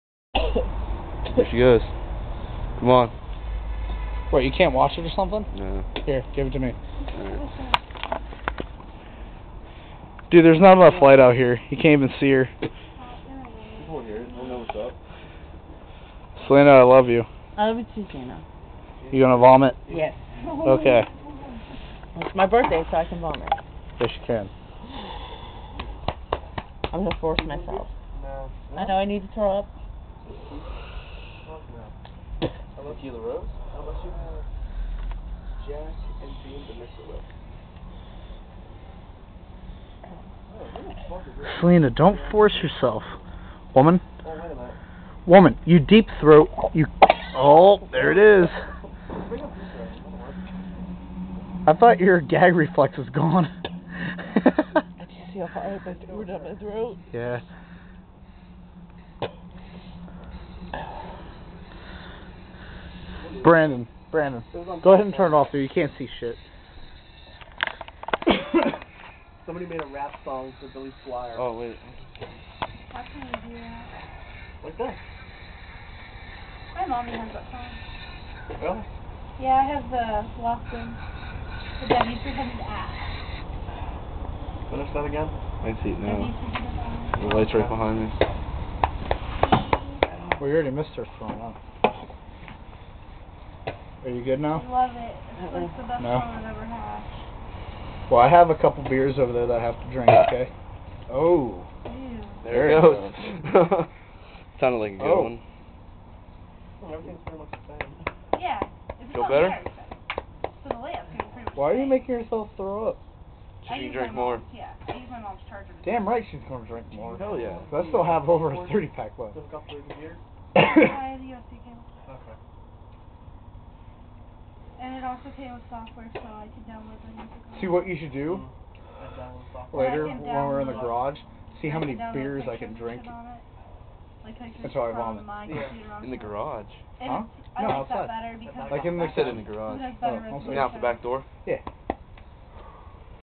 Celena Puking
Celena is puking after too much to drink at her 29th birthday party.